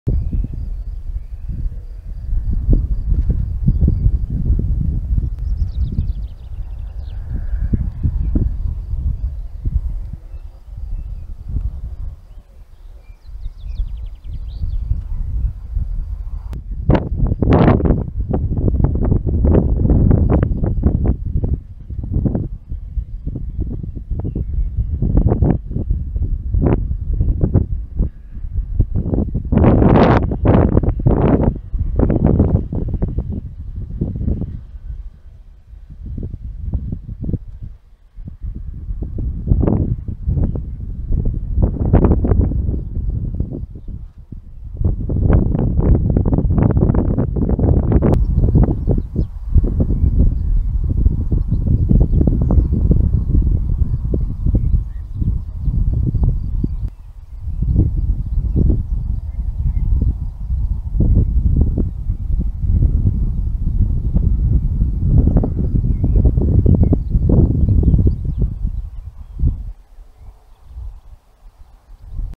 Modified 1.5 m Malay / Eddy kite
Plastic, tape, pine wood construction. 3-point bridle